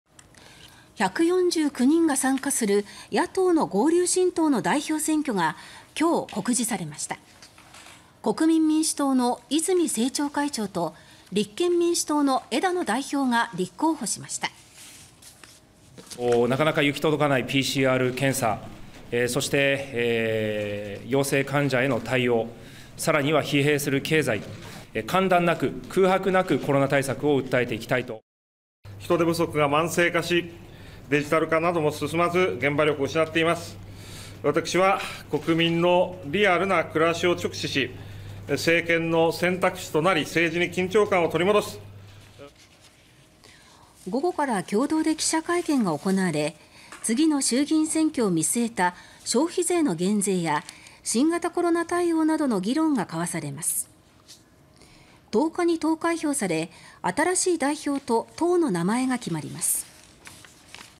“合流新党”代表選告示 10日に新代表と党名決まる(2020年9月7日)
149人が参加する野党の合流新党の代表選挙が7日に告示されました。国民民主党の泉政調会長と立憲民主党の枝野代表が立候補しました。

　国民民主党・泉政調会長：「なかなか行き届かないPCR検査、そして陽性患者への対応、さらには疲弊する経済。間断なく空白なくコロナ対策を訴えていきたい」
　立憲民主党・枝野代表：「（公的サービスでは）人手不足が慢性化し、デジタル化なども進まず、現場力を失っています。私は国民のリアルな暮らしを直視し、政権の選択肢となり、政治に緊張感を取り戻す」
　午後から共同で記者会見が行われ、次の衆議院選挙を見据えた消費税の減税や、新型コロナ対応などの議論が交わされます。10日に投開票され、新しい代表と党の名前が決まります。